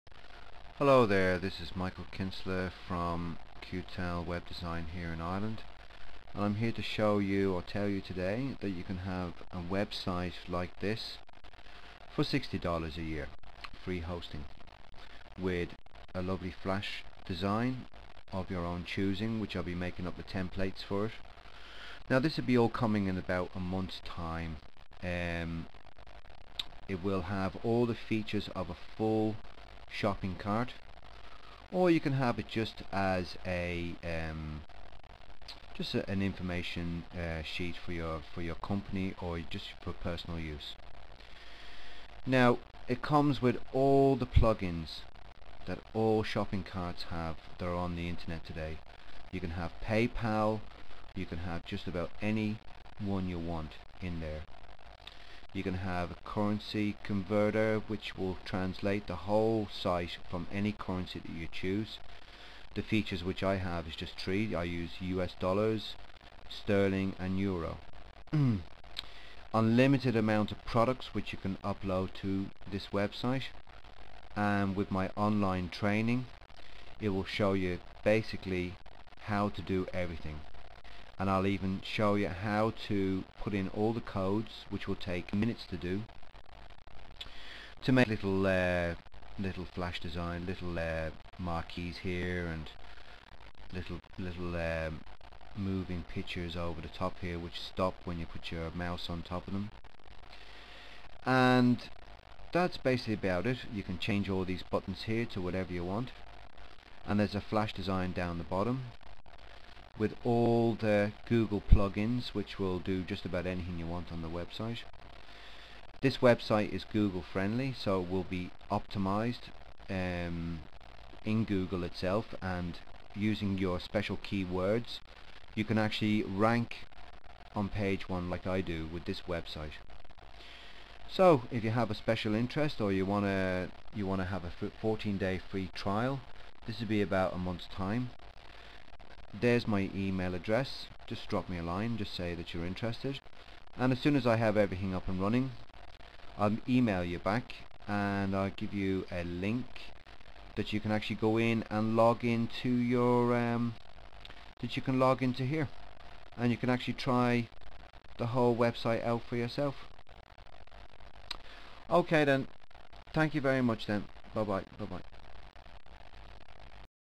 UK Ireland USA World Free web Sites Free Trial
looking for a web site with flash, 14 day free trial for this type of web site can be used fo just about anything and ranks very high in Gooole and other major search engines with video you could end up with a web

Can be used for dropshipping business shopping blog affiliates complete, full online video trainning for this web site and with a 14 free trail, no special computer trainning involved all you need to do is switch the computer on and watch michael kinsella work for you and just copy and learn.

Qtellwholesale Limited is 100% Irish